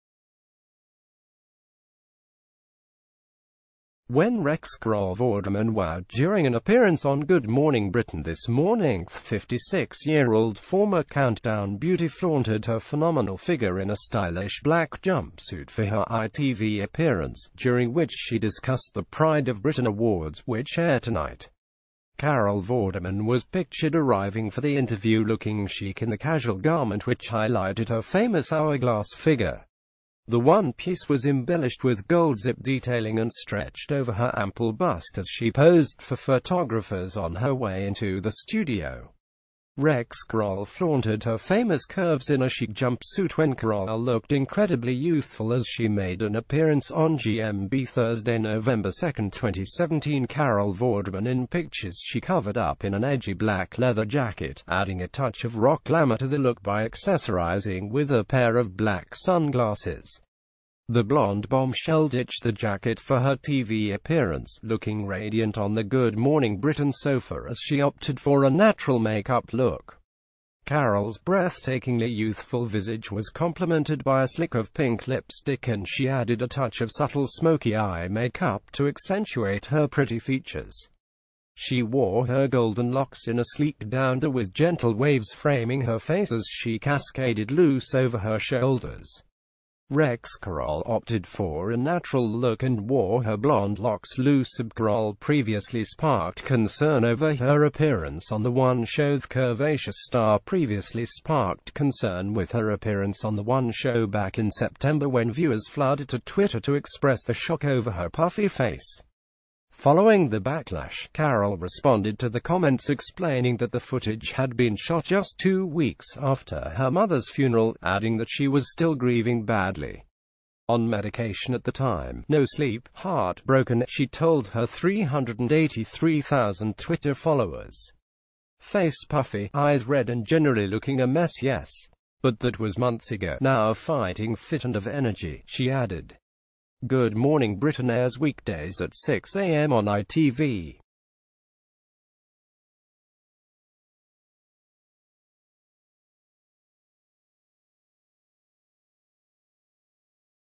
Carol vorderman looks incredibly youthful as she flaunts fantastic figure on gmb| Breaking News TV
WENN • REXCarol Vorderman wowed during an appearance on Good Morning Britain this morningThe 56yearold former Countdown beauty flaunted her phenomenal...